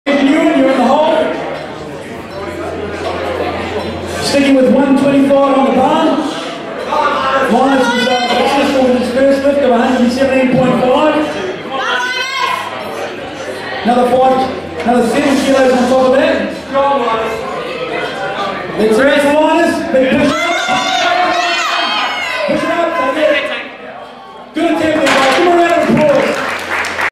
Linus Lim - Bench Attempt 2 - 125kg @ 80.7kg - 2013 Wild West Shootout
Unfortunately Linus unable to convert his second attempt, just got stick mid way.